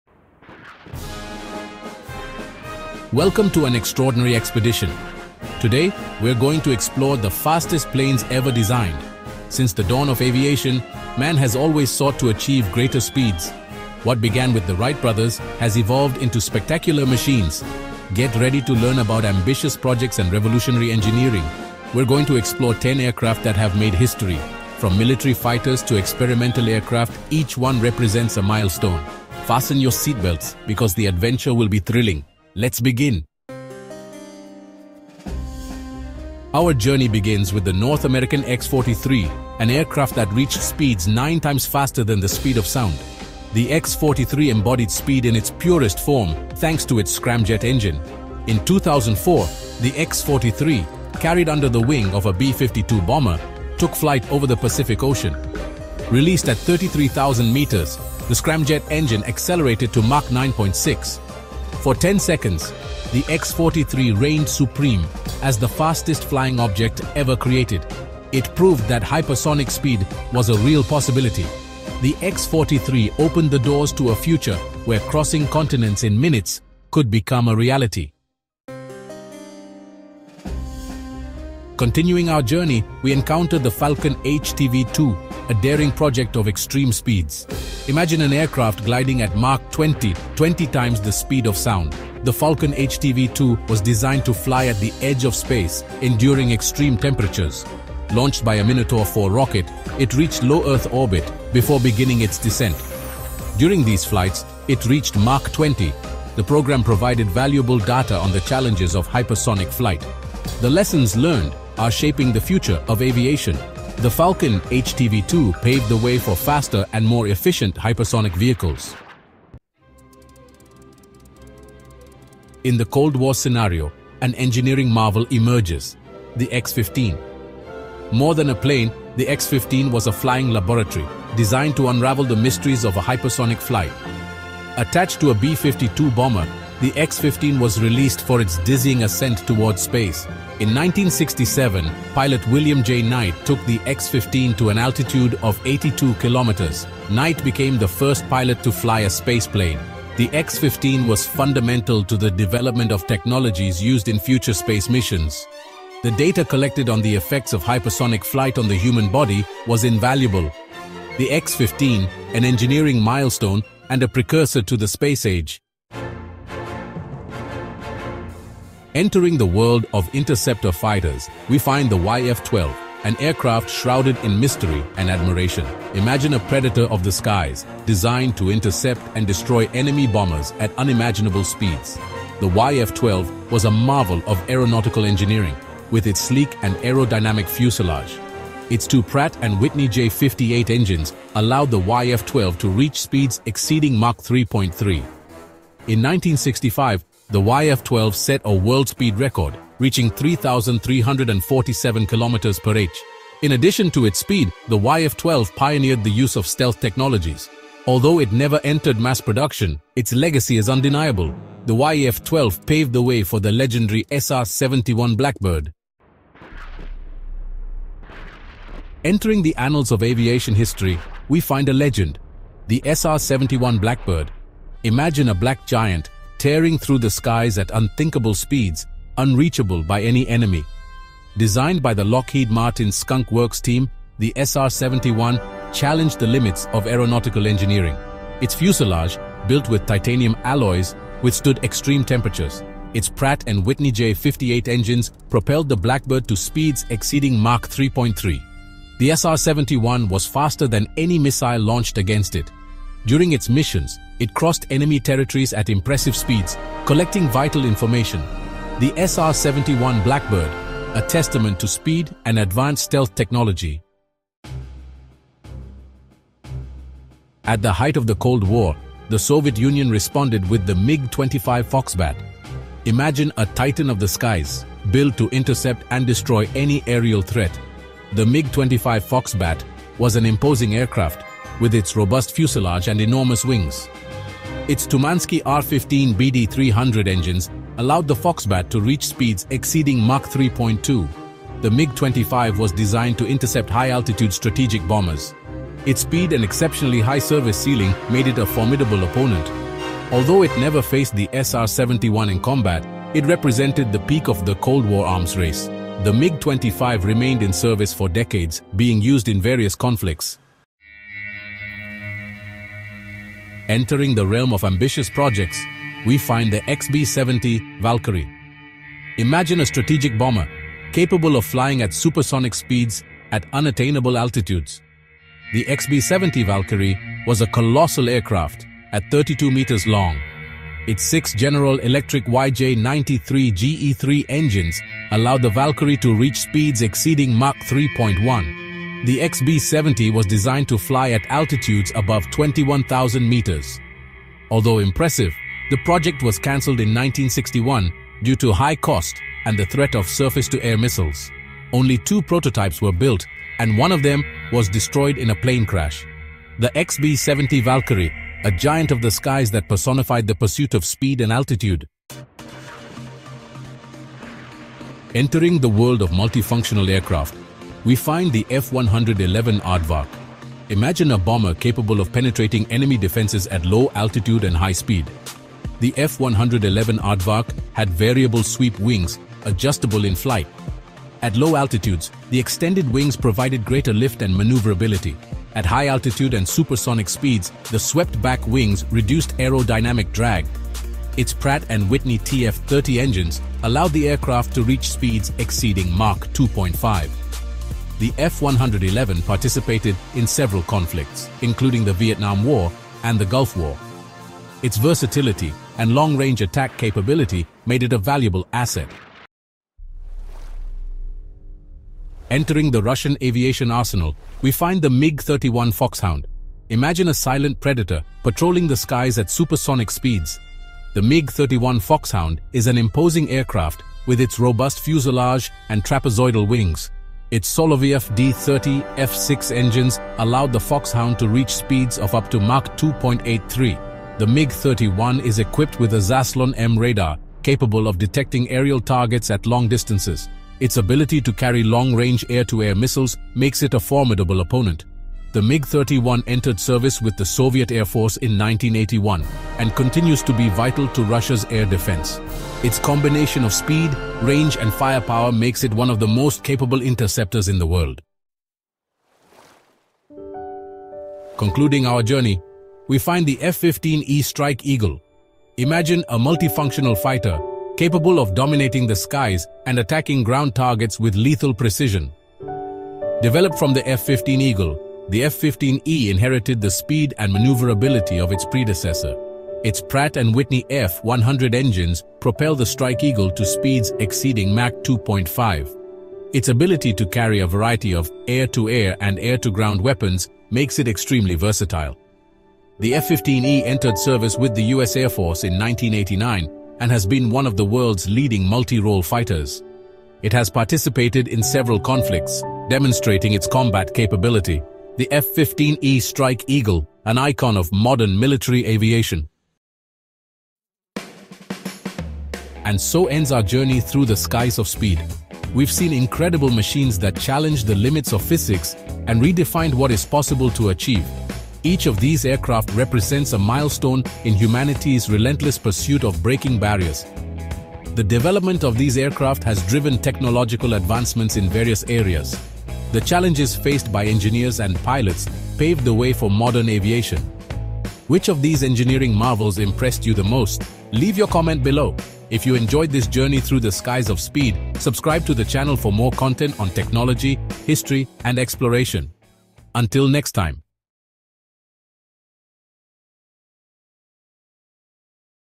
Supersonic Machines! The Planes That Broke All Speed Records!
🚀 Prepare for a mind-blowing journey through the skies! In this video, you will meet the 10 fastest planes in the world, revolutionary machines that have challenged the limits of speed and technology. From military fighters to hypersonic experiments, each of these planes has reached impressive speeds and changed aviation history forever!

🔥 What you will see in this video:
✈️ Supersonic fighters that have dominated the skies
🚀 Experimental planes that have pushed the limits of physics
⚡️ Machines capable of reaching unbelievable speeds!

Did you know that some of these planes can fly faster than a missile? Or that NASA has created aircraft that reach Mach 9.6? Find out how these true beasts of the air work and which one has reached the highest speed ever recorded!